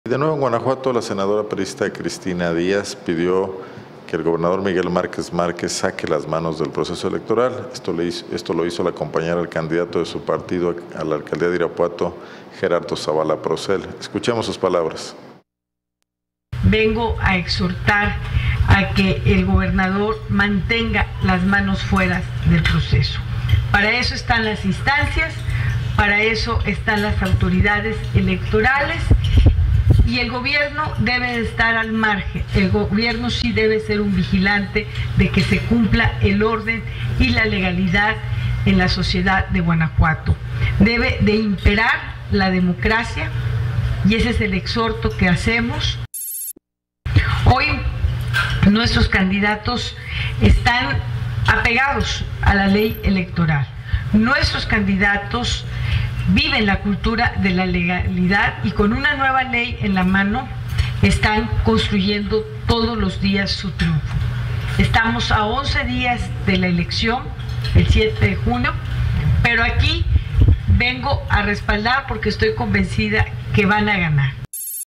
Se une Cristina Díaz al reclamo: ‘que Miguel Márquez saque las manos del proceso electoral’
La senadora comentó que los candidatos del PRI en el estado llevan a cabo su campaña bajo la ley electoral